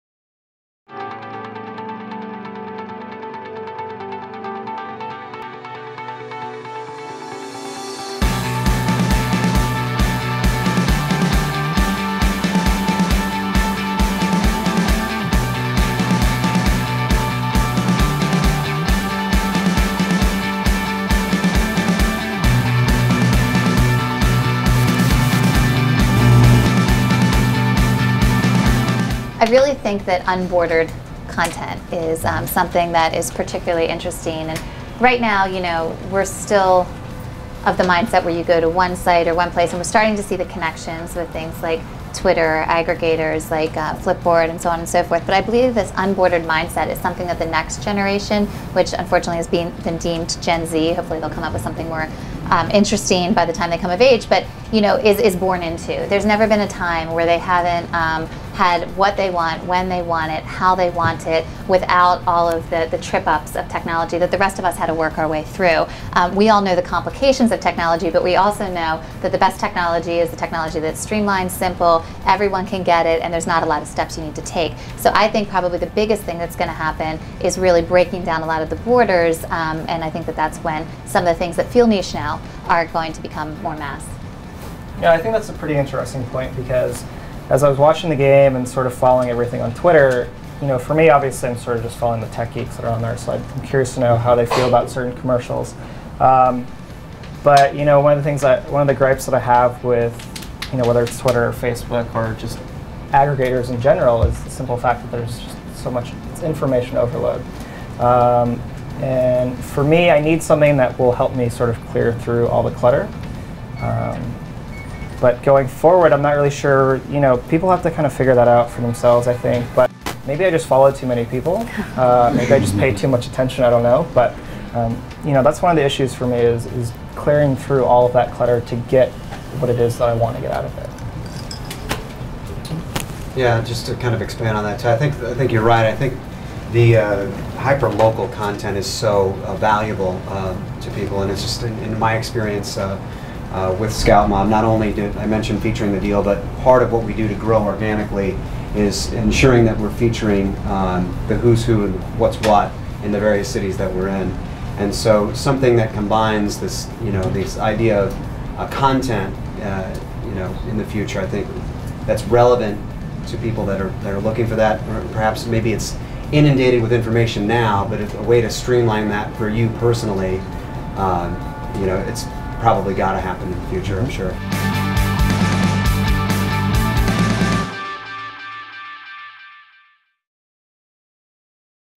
Mobile Meets Social: Converting Check-Ins to Check- Outs - 5
Panelists from Taylor's Social Media Week 2011 event discuss what consumers can expect next from location-based services and mobile marketing.